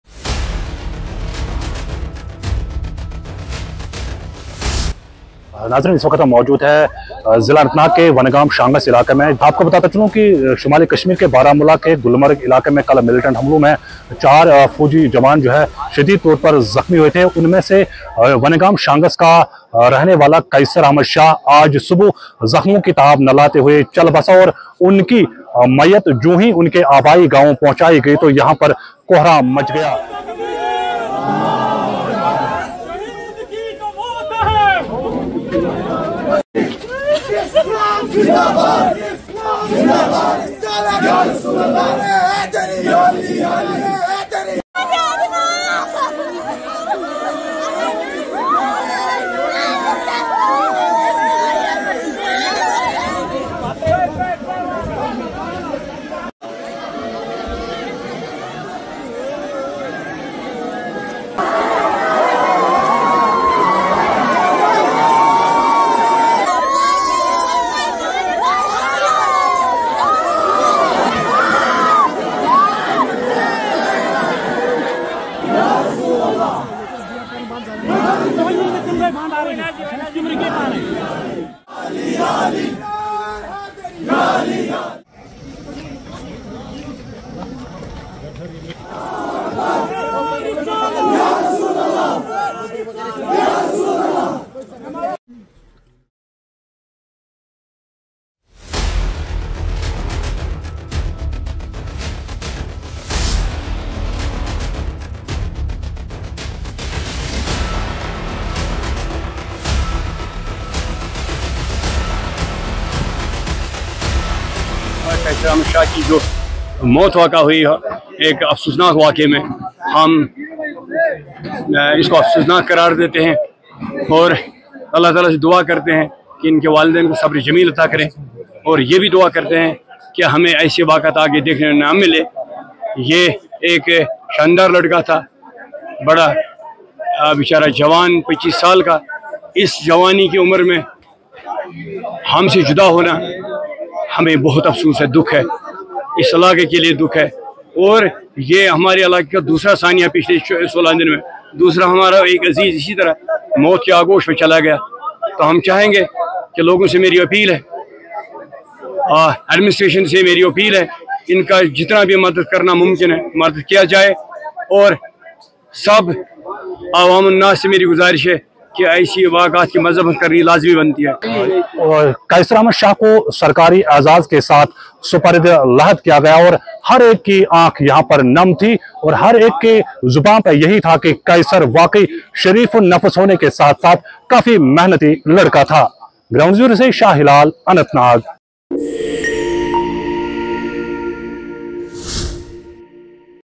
Braveheart Kaisar Ahmed Shah's Mortal Remains Reach Native Village || AMC
Watch: Braveheart Kaisar Ahmed Shah's Mortal Remains Reach Native Village